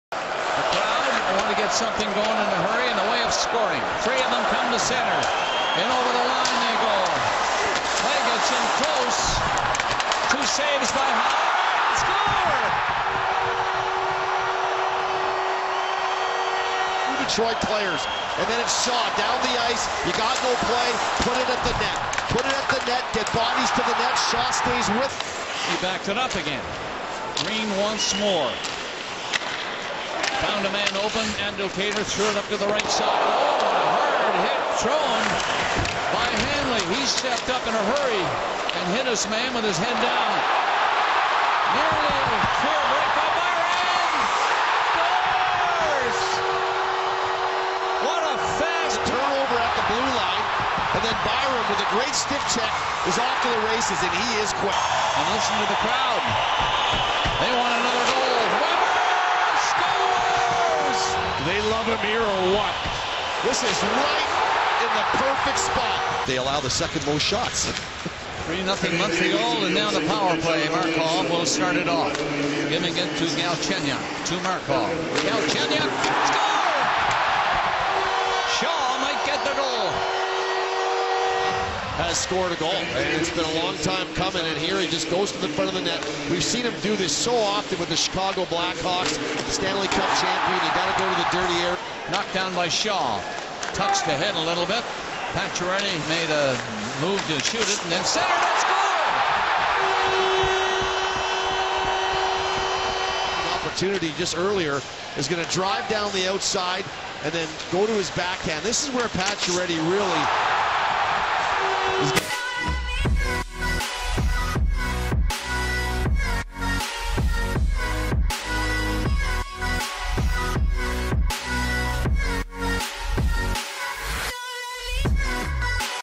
Montreal Canadiens vs Detroit Red Wings | Season Game 15 | Highlights (12/11/16)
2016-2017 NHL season
Watch every goals and highlights from November 12 featuring the Montreal Canadiens and the Detroit Red Wings!

GOALS
Danault(5)
Byron(5)
Weber(6)
Shaw(2)
Pacioretty(3)

I'm always open to suggestion for future video!